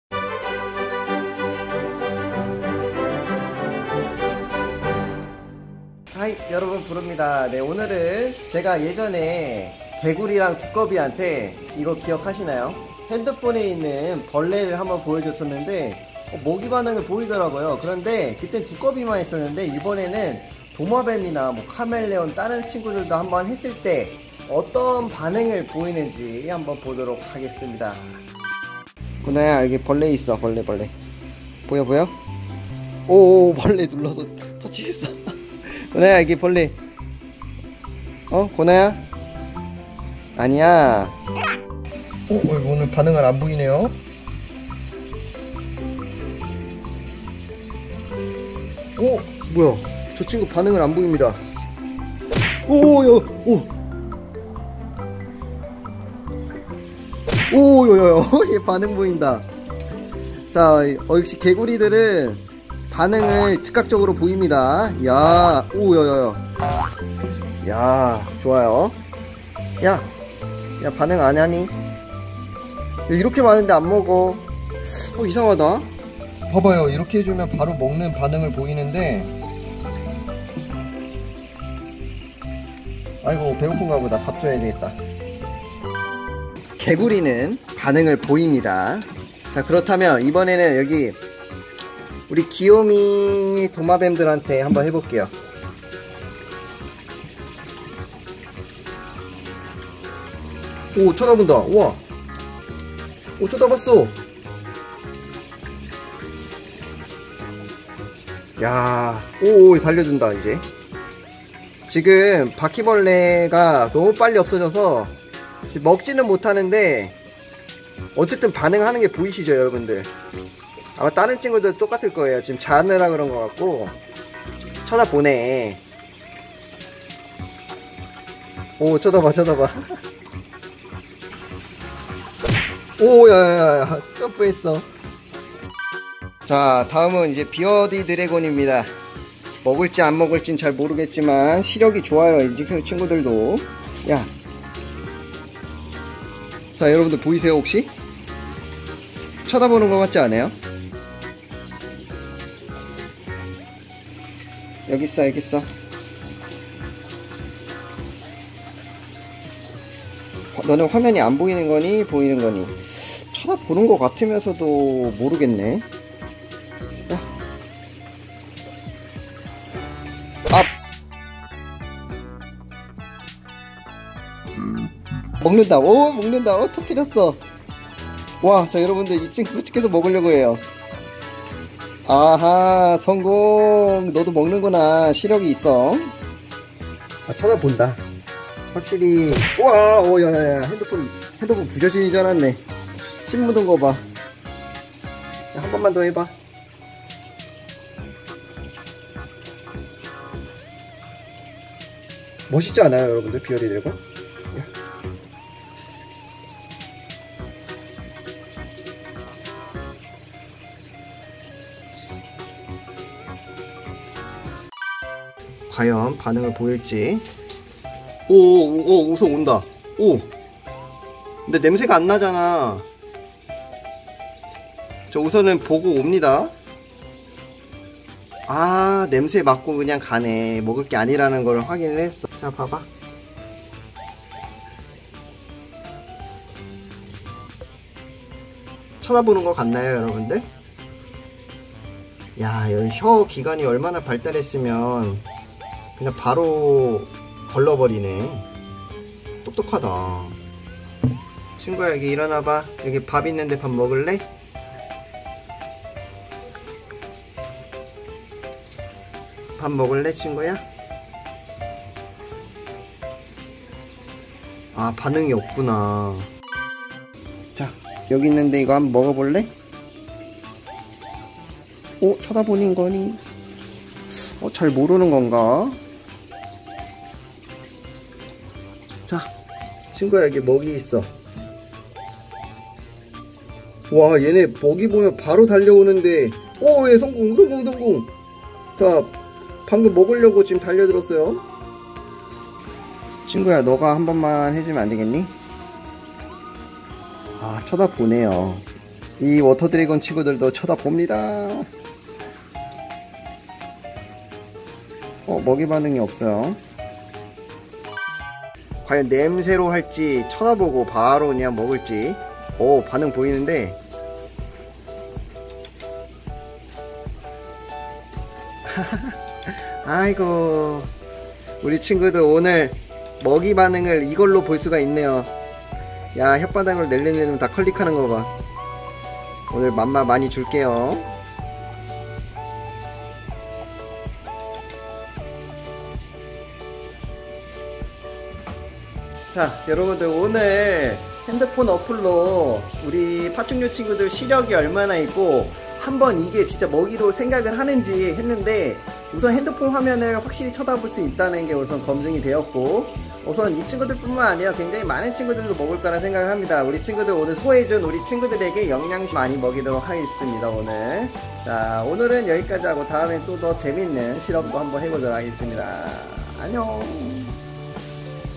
도마뱀에게 핸드폰벌레 보여줘봤다. [정브르]
저도 예전에 두꺼비에게 한적이 있는데 그때는 잘 먹어주었습니다만!
파충류들에게 해본적은 없거든요!  과연 어떨지~~???

예스24 "크리피 구미" 증정 이벤트!

인터파크 "동물친구 펀트 증정"!

교보문고 "정브르 사육카드" 증정 이벤트!

알라딘 동아오츠카 "오라떼" 증정 이벤트!